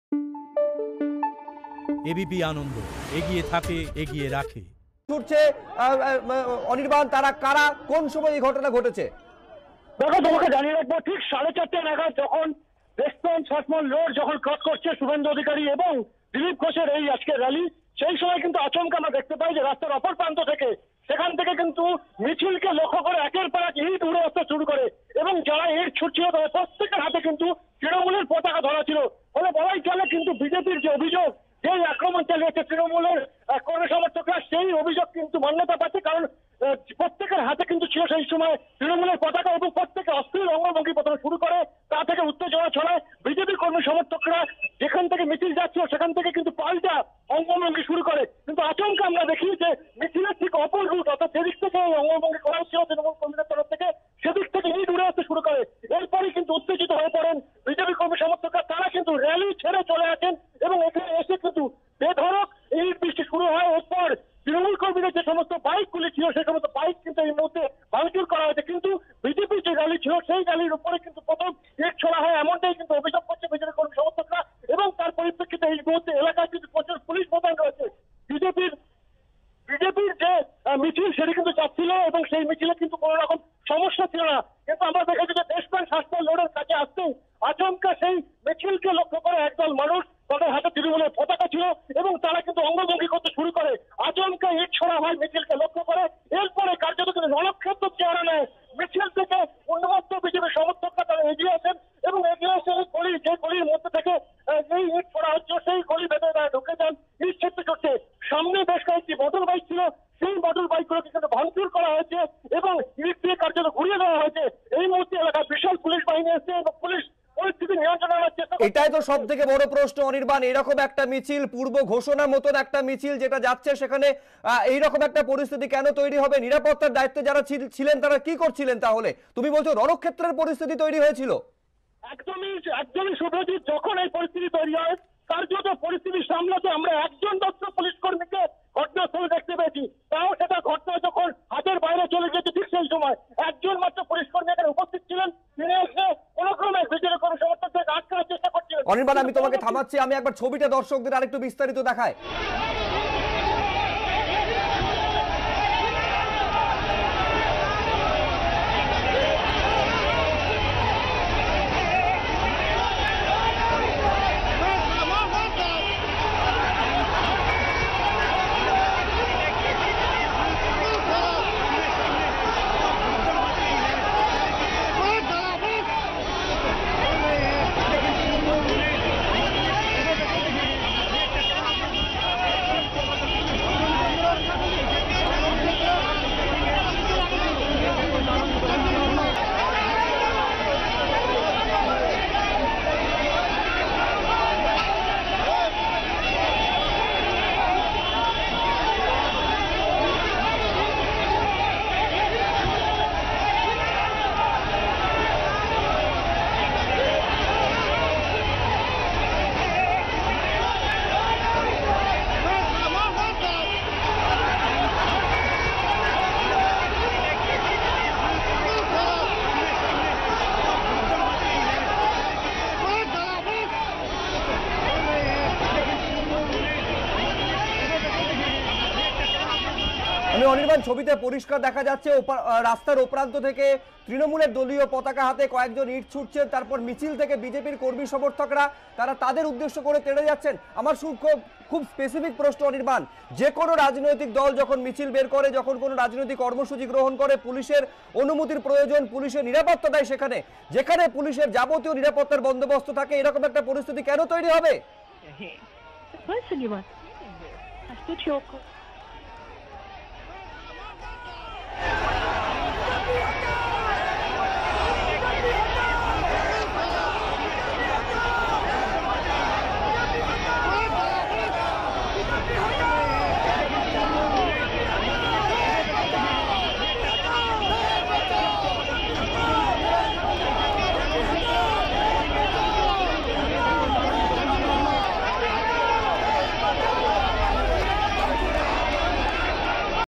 দক্ষিণ কলকাতায় BJP-র মিছিলে ইটবৃষ্টি, হামলাকারীদের হাতে তৃণমূলের পতাকা ছিল, অভিযোগ গেরুয়া শিবিরের
সোমবার দক্ষিণ কলকাতায় শুভেন্দু অধিকারী (Suvendu Adhikari) ও দিলীপ ঘোষের (Dilip Ghosh) মিছিলে ইটবৃষ্টি। বিজেপি কর্মীদের অভিযোগ, টালিগঞ্জ থেকে মিছিল শুরু হওরা পর দেশপ্রাণ শাসমল রোডের কাছে আসতেই মিছিলের দিকে একের পর এক ইট উড়ে আসতে শুরু করে। যারা ইট ছুঁড়ছিল প্রত্যেকের হাতে তৃণমূলের পতাকা ছিল। মিছিলের দিকে তাকিয়ে অশ্লীল অঙ্গভঙ্গি করা হয়। এর পরেই উত্তেজিত বিজেপি কর্মীরা মিছিল ছেড়ে গিয়ে তৃণমূল কর্মীদের মোটরসাইকেল ভাঙচুর করে। এলাকায় প্রচুর পুলিশ মোতায়েন রয়েছে।